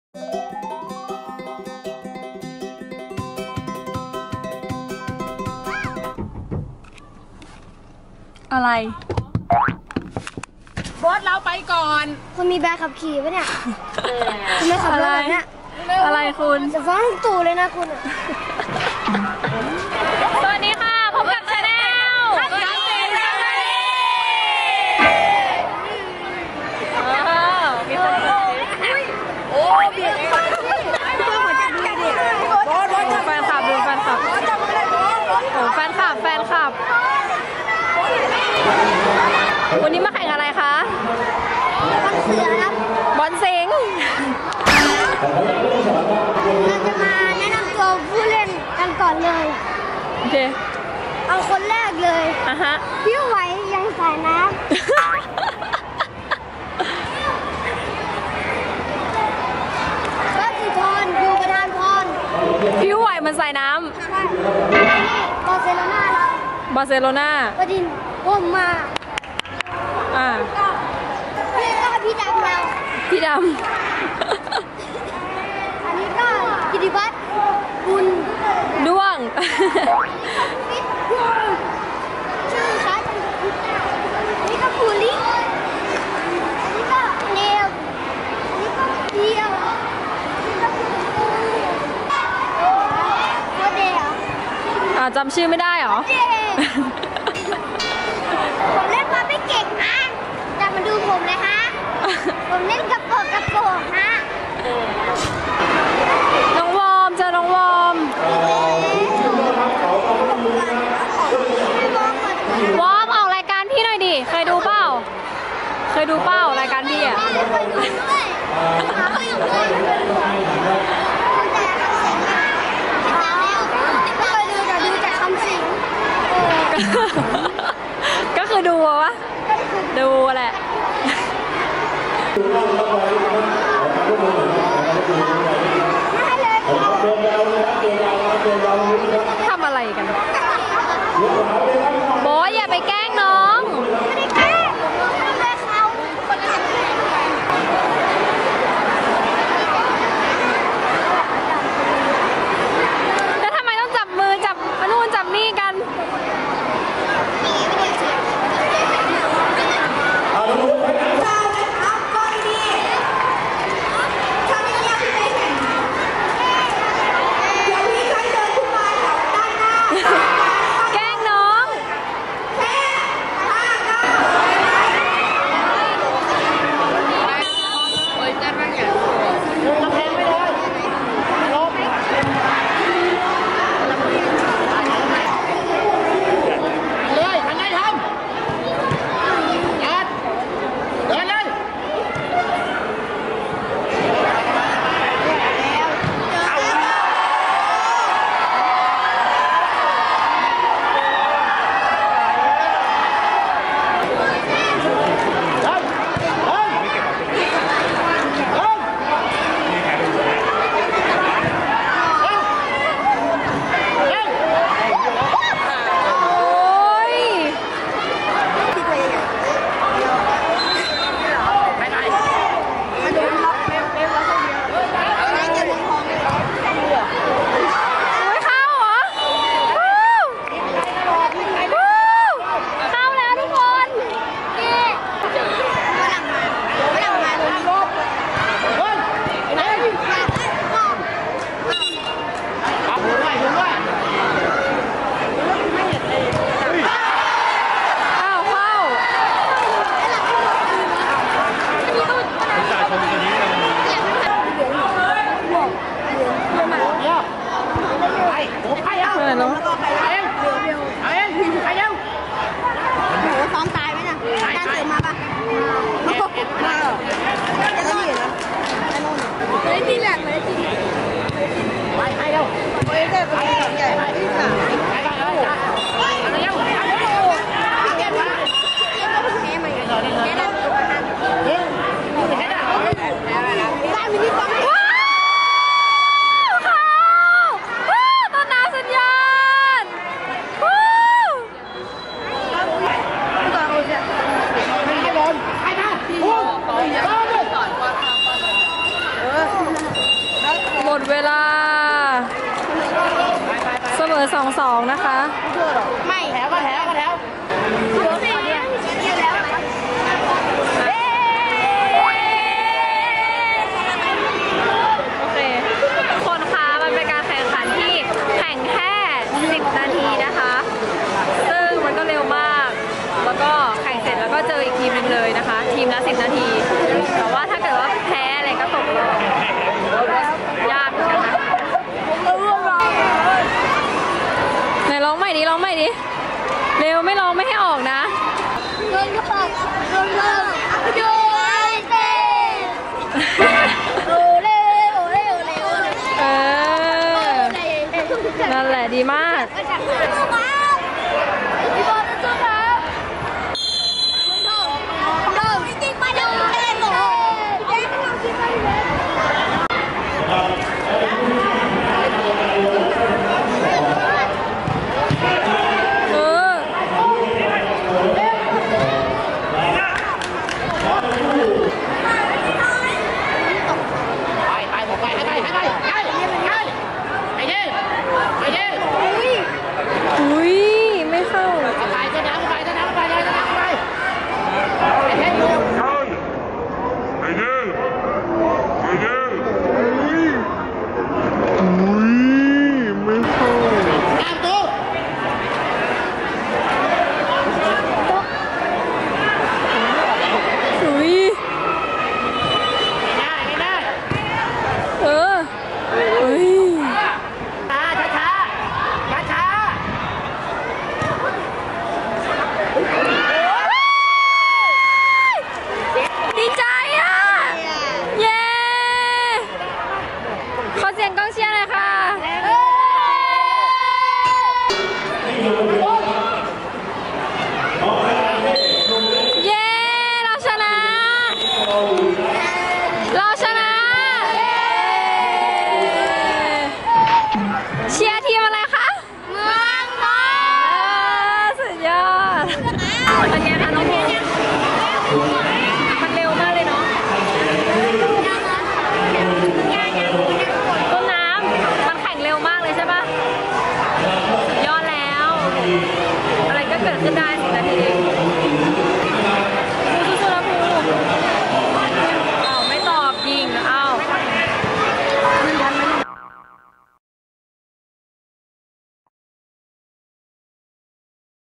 KAMSING FAMILY | โบ๊ท กับน้องวอร์มอัพ แข่งสนามเดียวกัน คิดถึงกันหนักมาก!!
โบ๊ท ภูวรักษ์ คำสิงห์ แข่งฟุตบอล บอลสิงห์ ในนามทีม SCG เมืองทอง ยูไนเต็ด เจอน้องวอร์มอัพ เคยร่วมรายการ Super10ด้วยกัน นานๆเจอที คิดถึงกันมาก!! แกล้งกันซะเลย